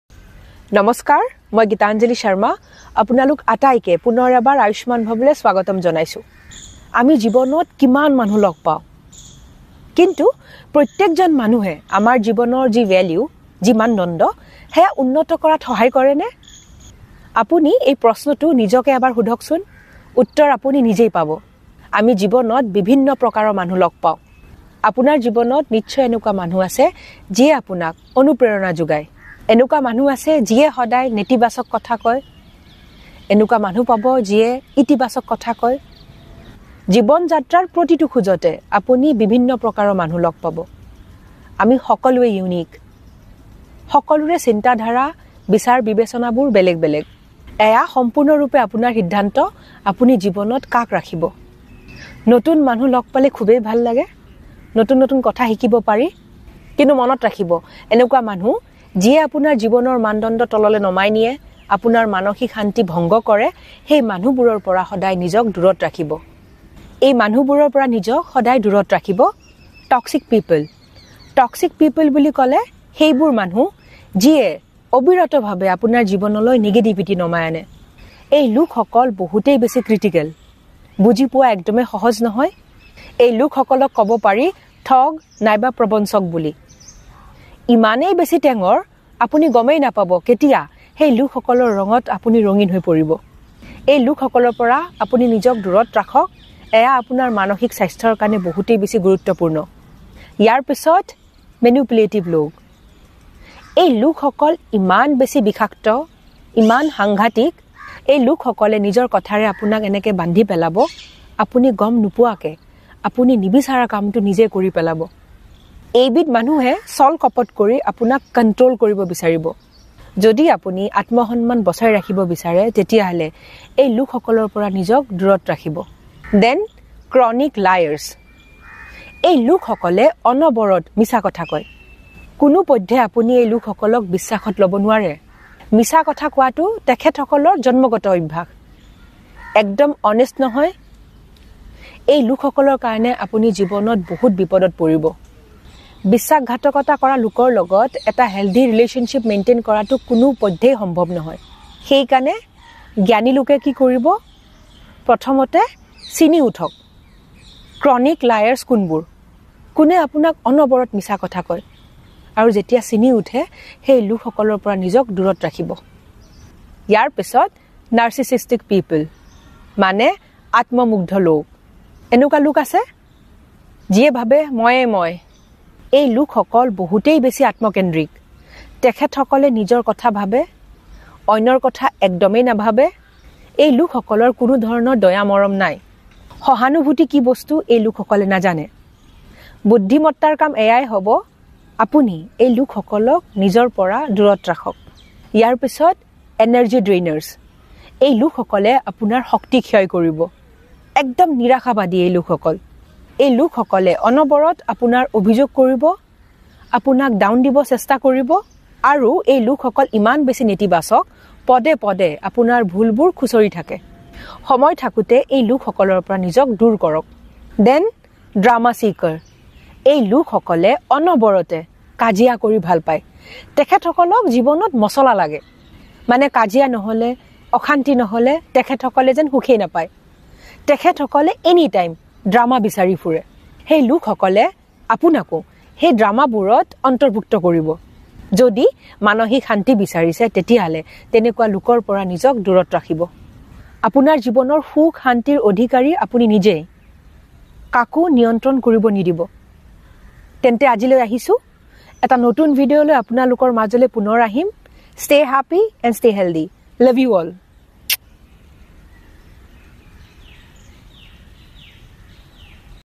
Keep A Distance From These People | Assamese Motivational Video
Keep A Distance From These People Assamese | Assamese Motivational Video | Powerful Motivation In Assamese | Assamese Knowledge | Assamese Motivation

🍁About this video:
Keep a distance from these people so that you can maintain your mental peace.

Practice Daily!
Stay Blessed! Stay Blissed!